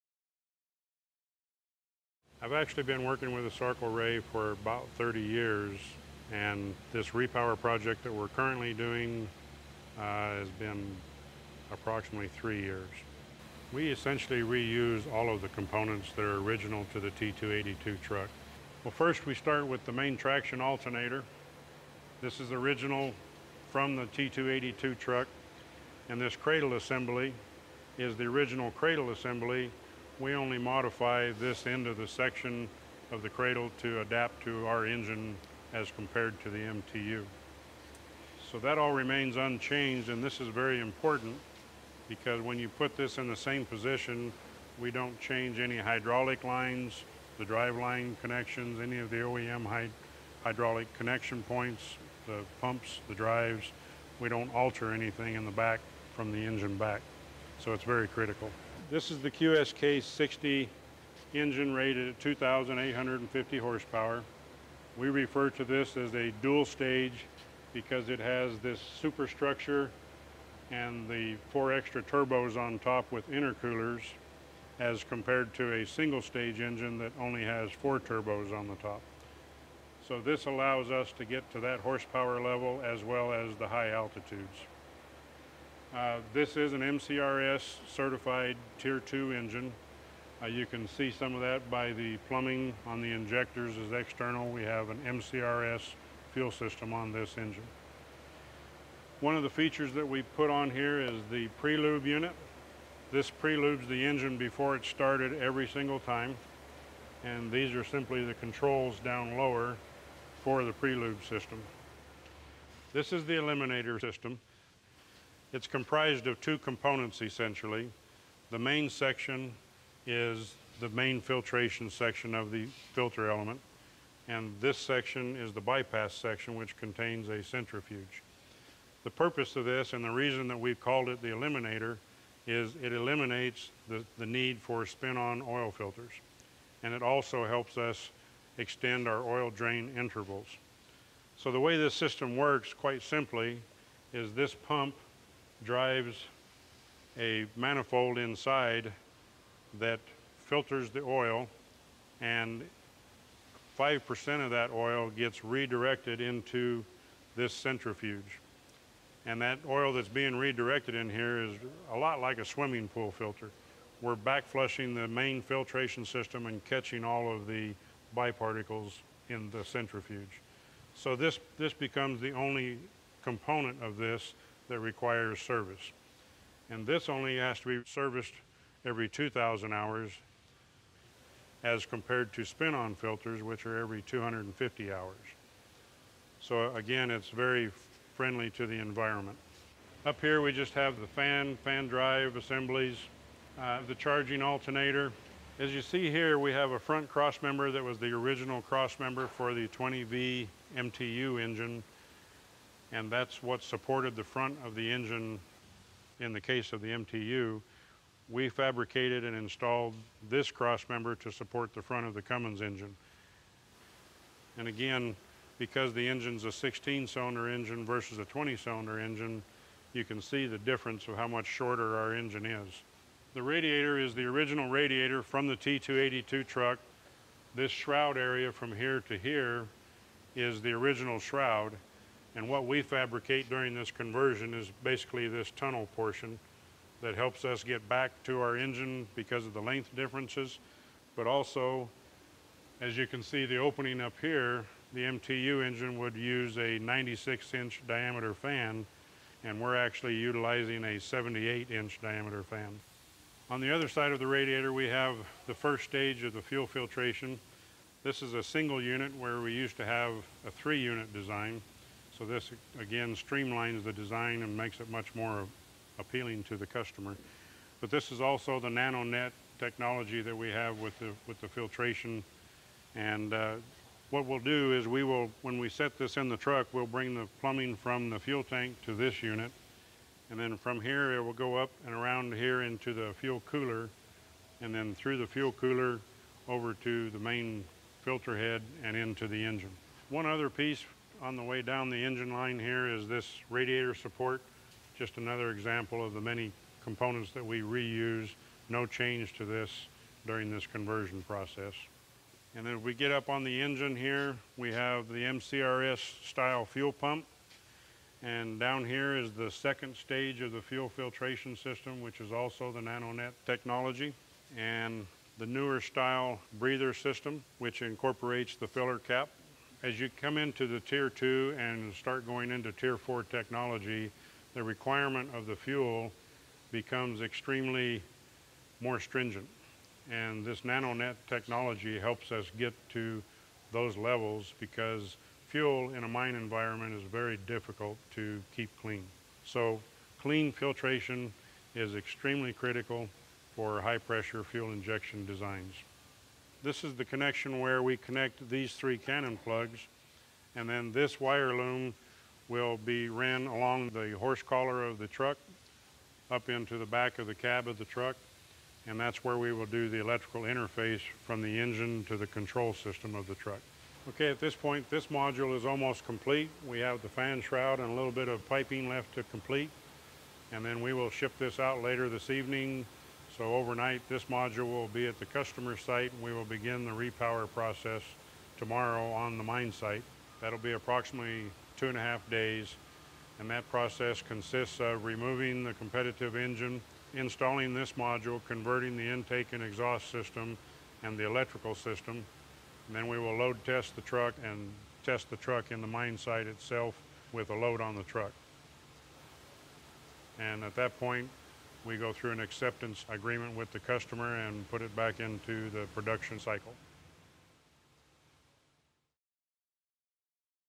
Cummins Repower Walkaround with ASARCO Ray Mine
When ASARCO Ray Mine needed engine replacements for their Liebherr T 282B 400-ton haul trucks, a Cummins team led by Brad Scow, General Manager – Mining, offered ASARCO a unique proposal. In this video, Scow shows the details of the repowered engine.